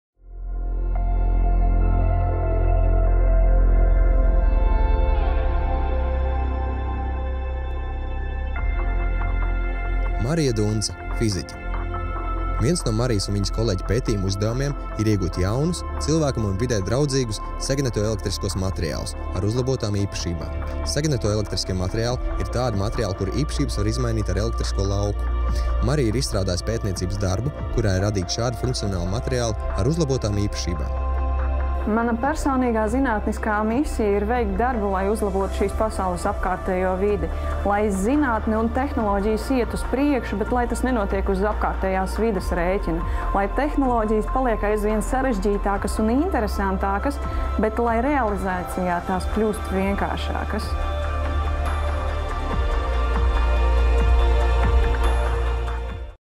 Physicist Marija Dunce (Latvia)
In the framework of cooperation with the Ministry of Education and Science of the Republic of Latvia communication agency A.W.Olsen & Partners has developed a series of video stories about outstanding Latvian scientists. That was a part of the project "Scientists of Latvia. Calendar 2019” ZinātneLatvijai. The agency provided full video production, starting with establishing the concept, interviewing the heroes, and ending with filming, editing and post-production.